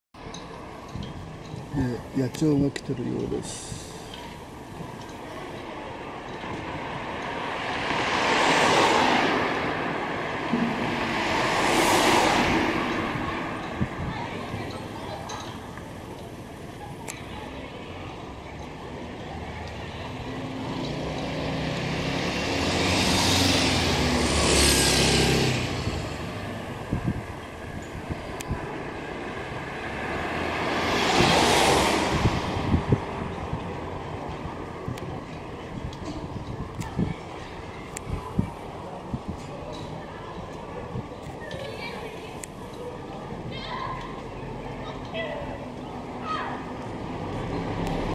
花の蜜とメジロ
桃の節句が近づき暖かくなってきましたが花にメジロが来ていました。食性は雑食ですが、花の蜜や果汁を好み、育雛期には虫なども捕食します。蜜を吸いに来たメジロですが目のまわりが白いので目白という名前がつきました。学校を欠席した子供向けに実験観察動画をつくっていますが授業の予習復習用などにも活用して下さい。